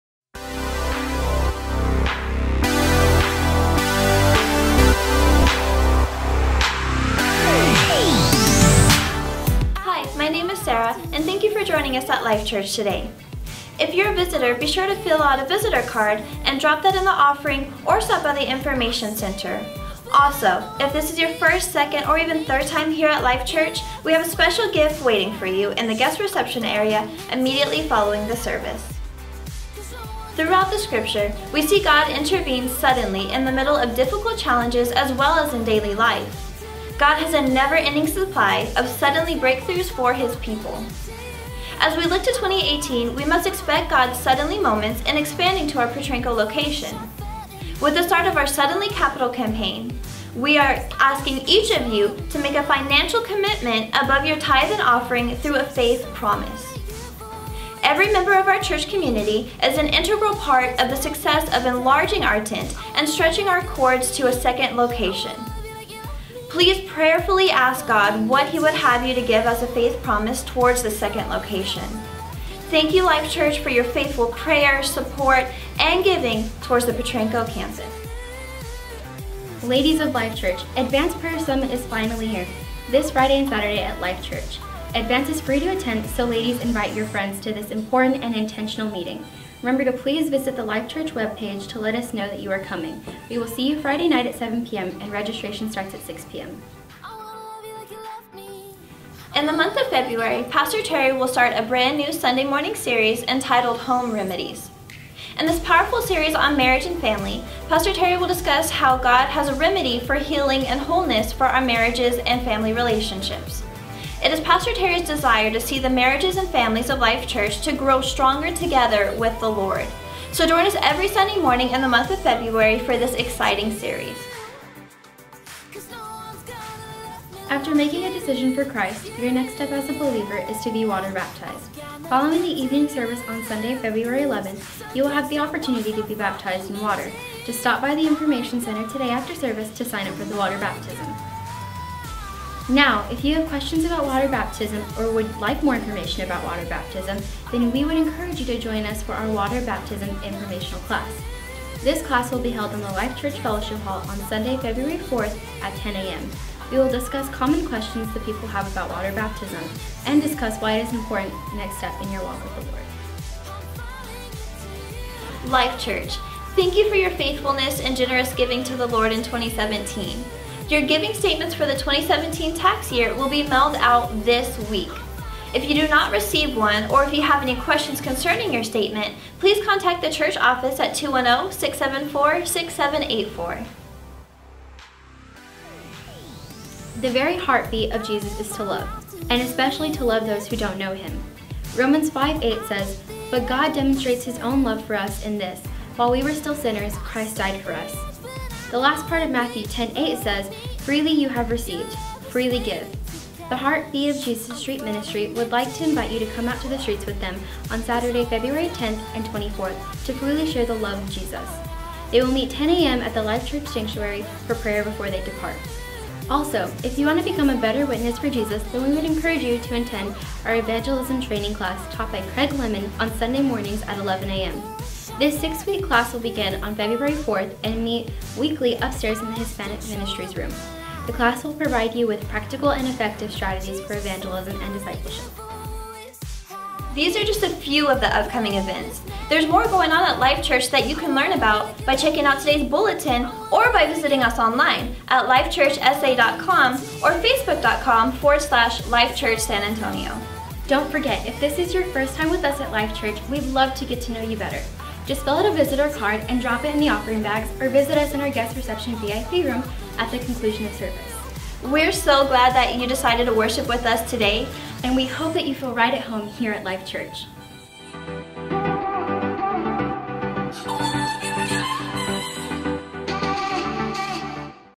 Life Church Announcements Jan. 28th
Watch our weekly video announcements to see all the upcoming events happening at Life Church San Antonio